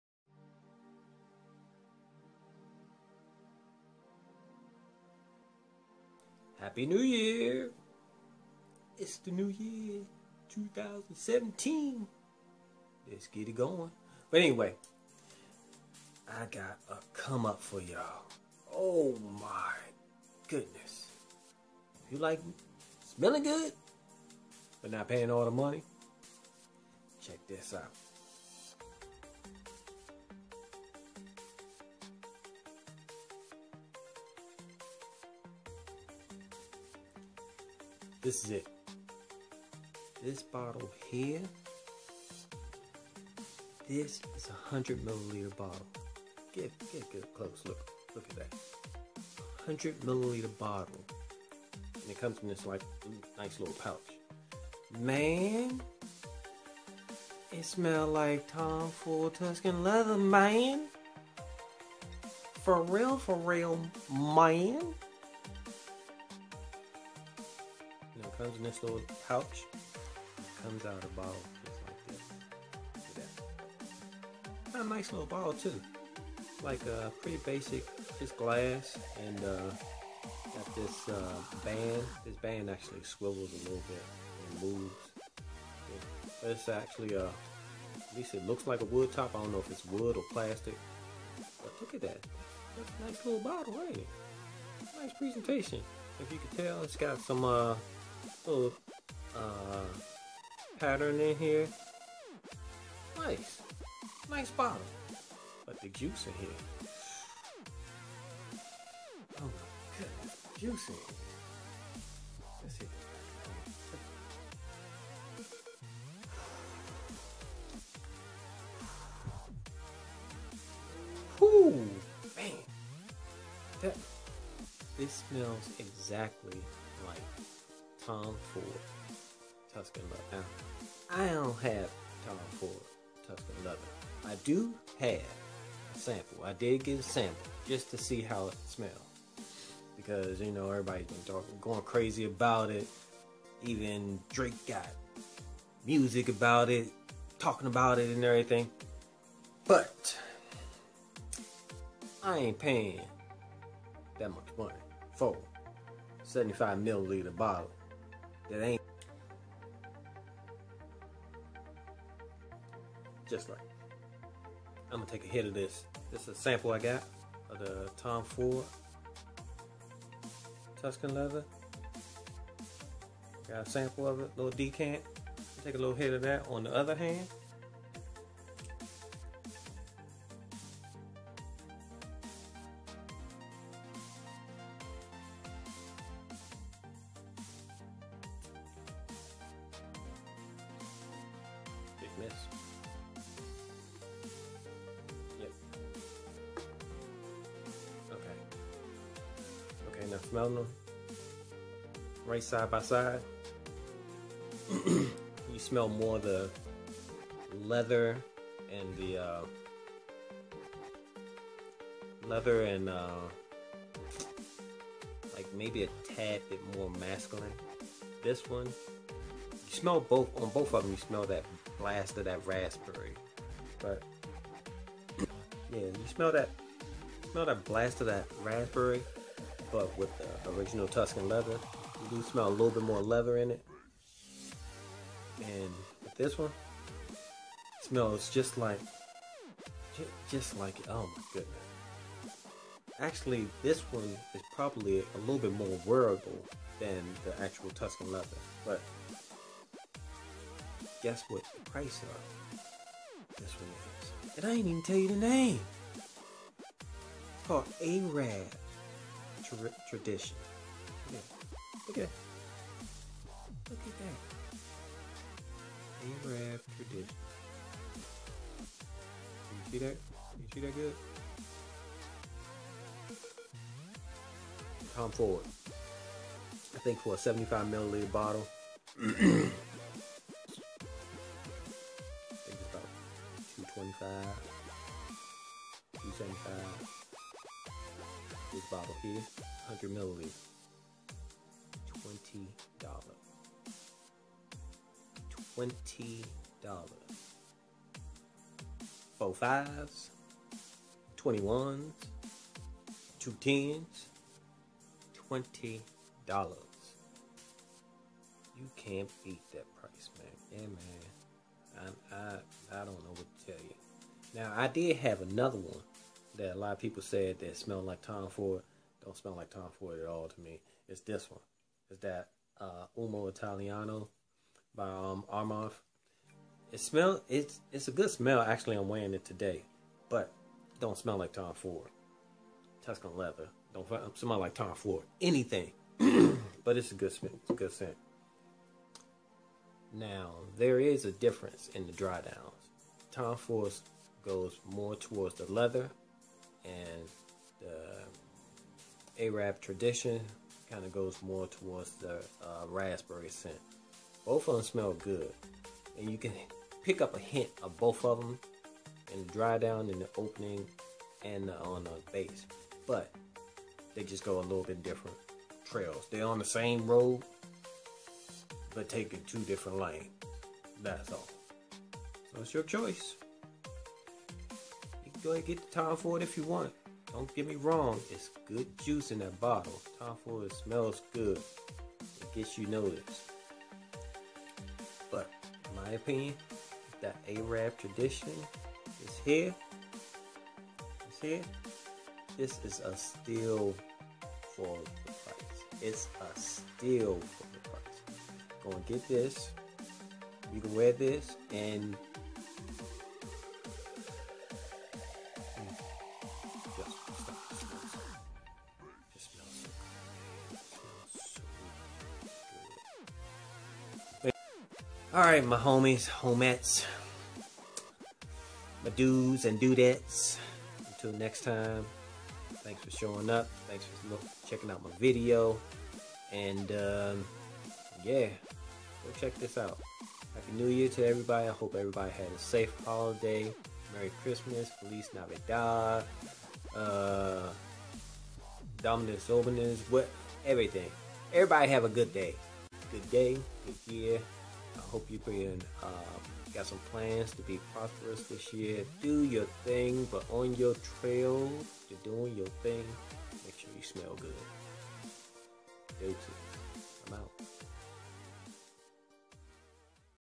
Arab Tradition by Nabeel (Tom Ford-Tuscan Leather Clone)
This is my take of this awesome fragrance. The similarities are close with a major difference in the prices.